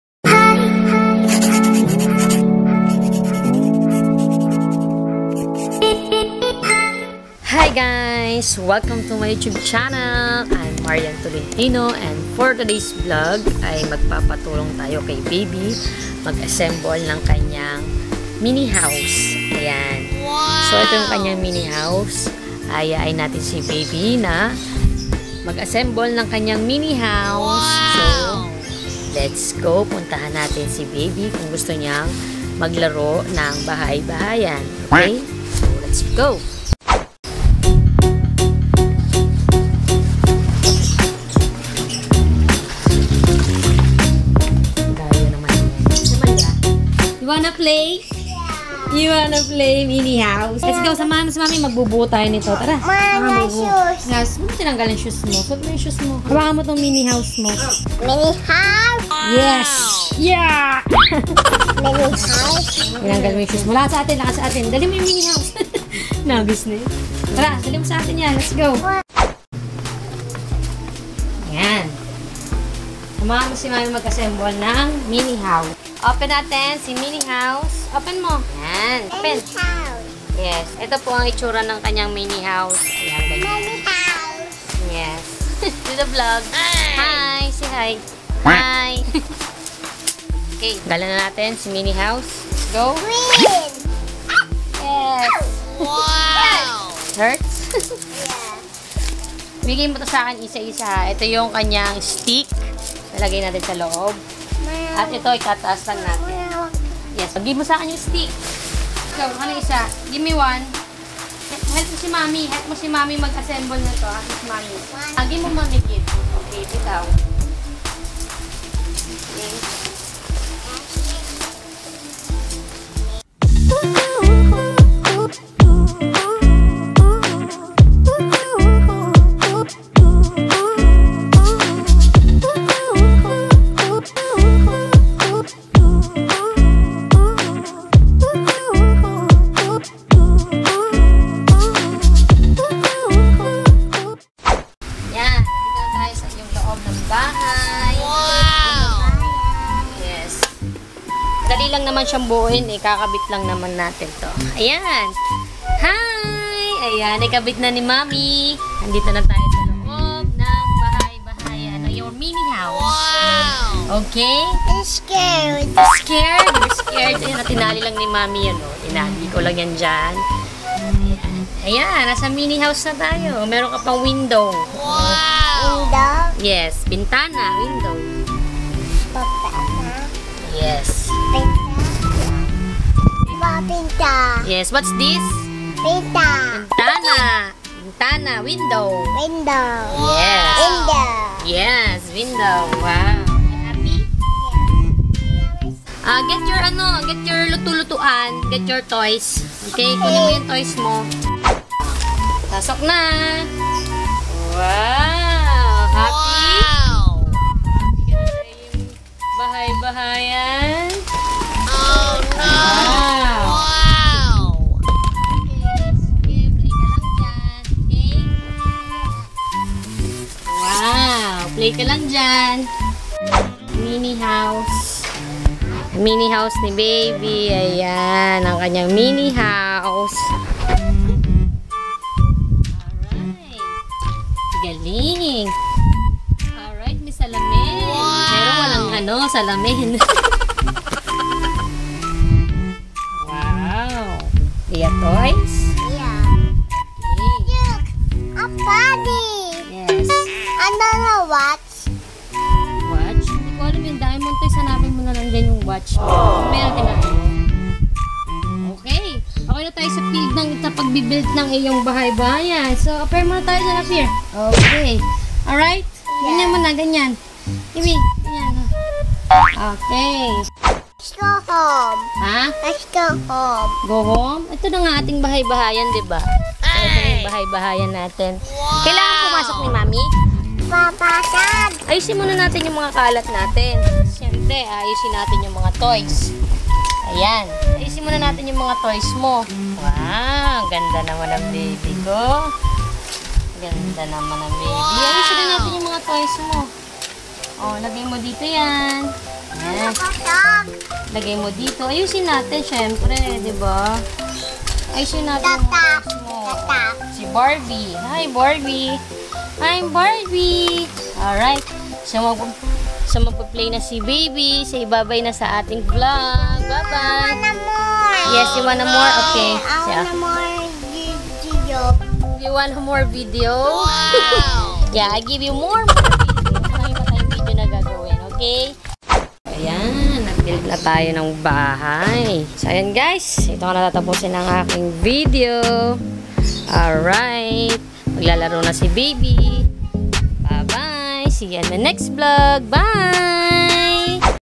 BAHAY- BAHAYAN | MINI HOUSE
BAHAY BAHAYAN OR MINI HOUSE, simple bagay na nakakapag pasaya sa kanya,binubuo ko ito kapag gusto nyang maglaro.

📍PREVIOUS VIDEO